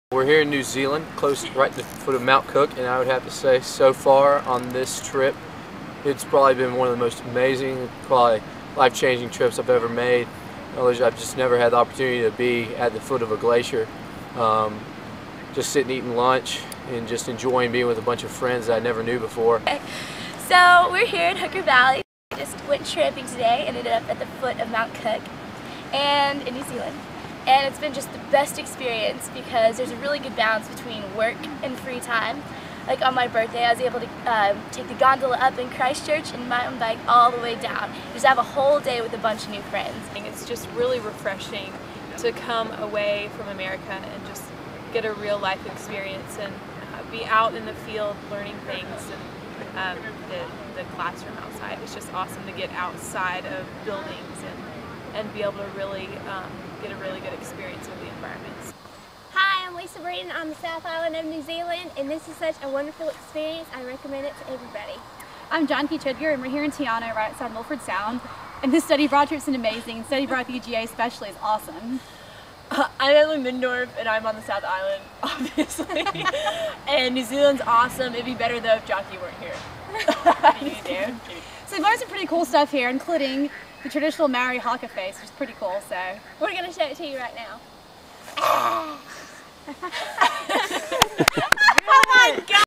kiwitalkstream.mov
Students in the University of Georgia Studies Abroad program in New Zealand discuss its value to them.